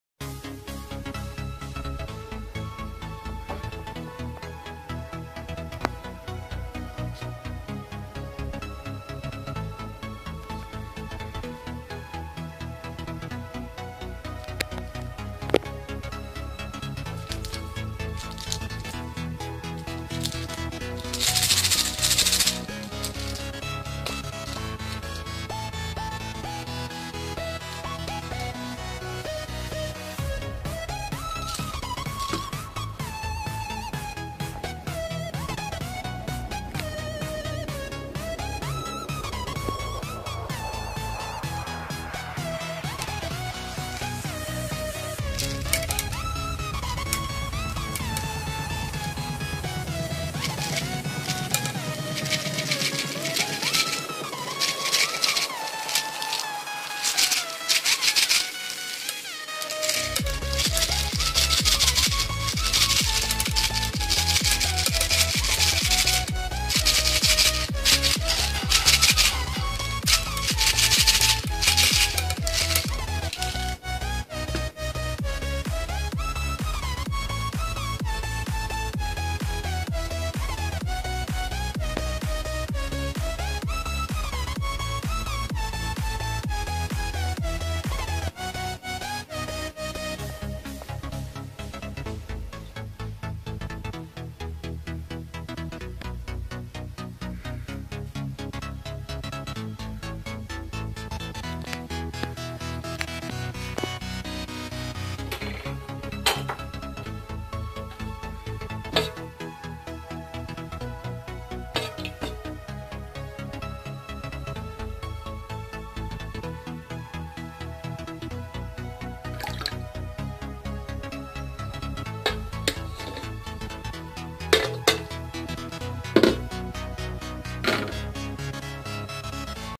Plastićne kuglice za pištolj otopile se u vrelu vodu (Experiments)
Ako vam se svideo ovaj video Lajkujte ga i prijavite se na kanal